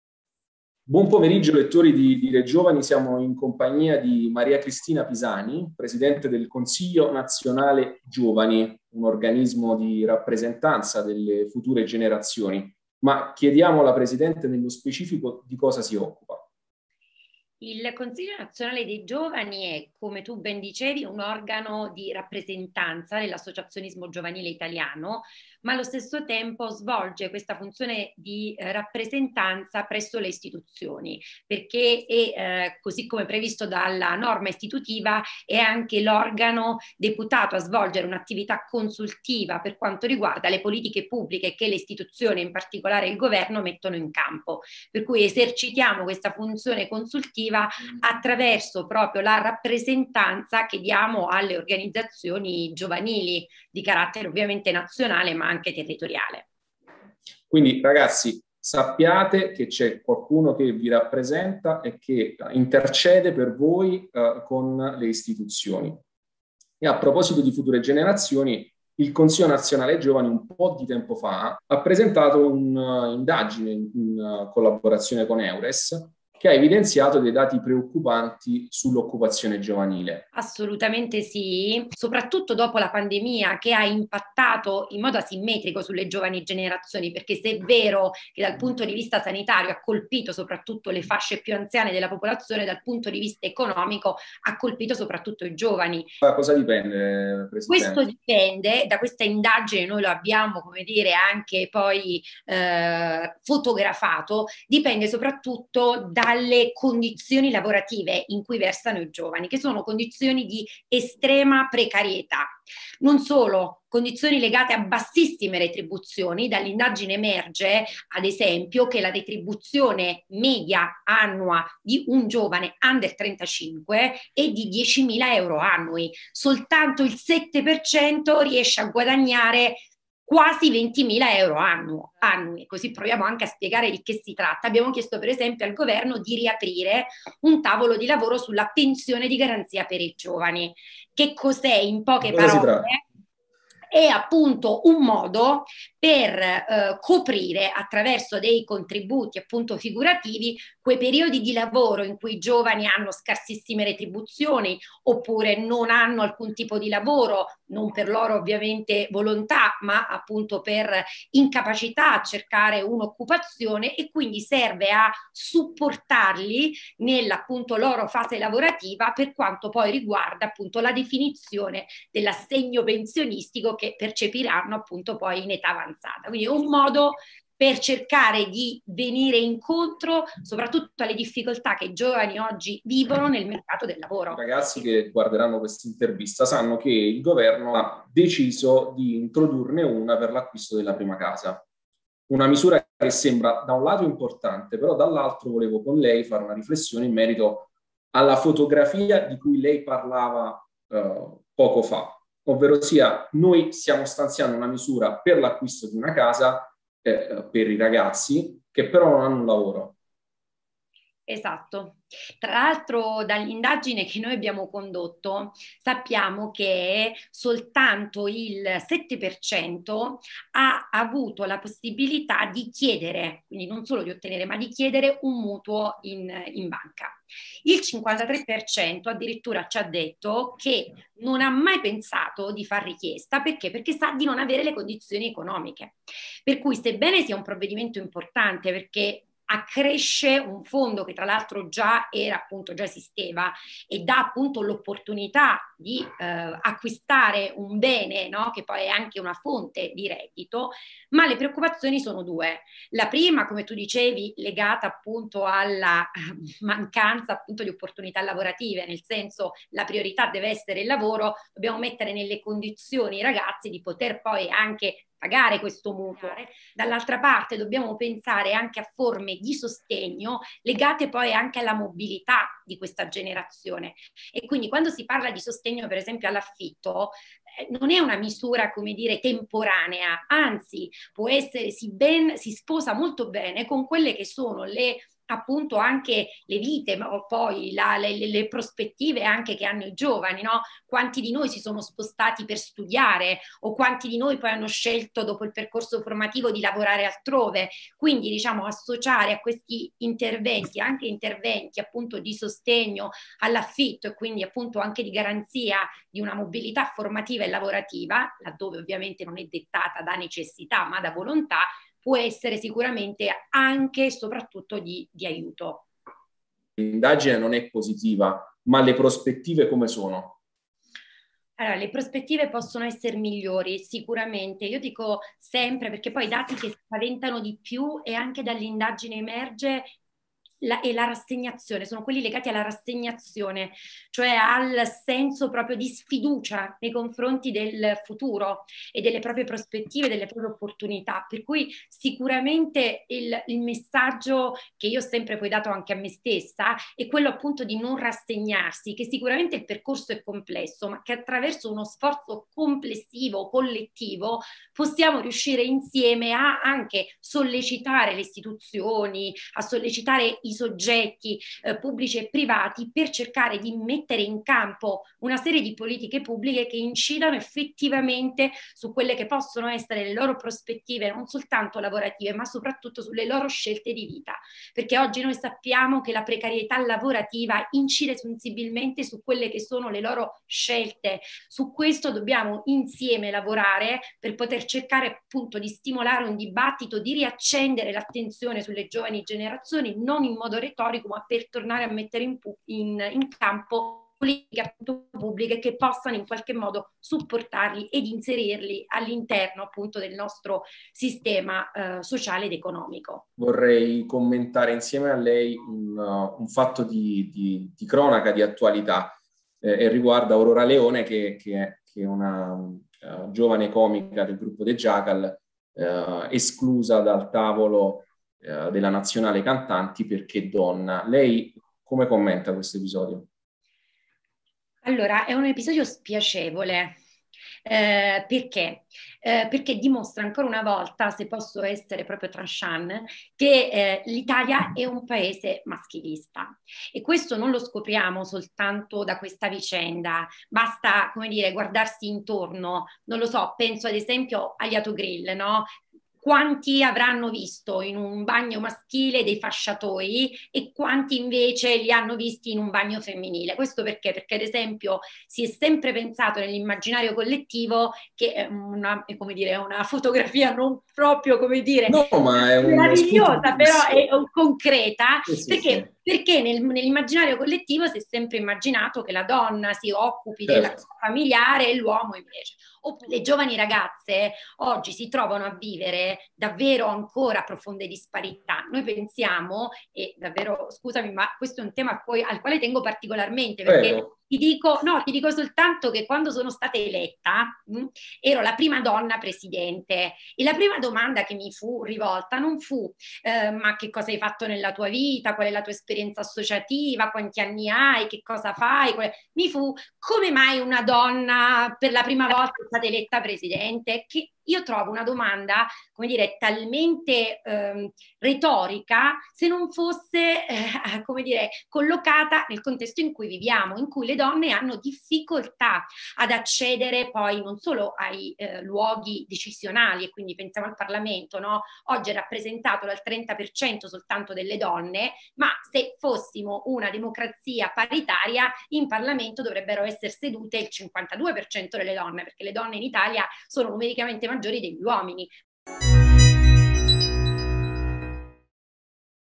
Intervista al presidente del Consiglio nazionale giovani
Il Consiglio Nazionale Giovani è un organo di rappresentanza dell’associazionismo giovanile italiano che svolge un importante compito di confronto e dialogo con le Istituzioni. La presidente Maria Cristina Pisani, eletta nel 2019, sembra avere le idee chiare su quali siano le priorità per le future generazioni.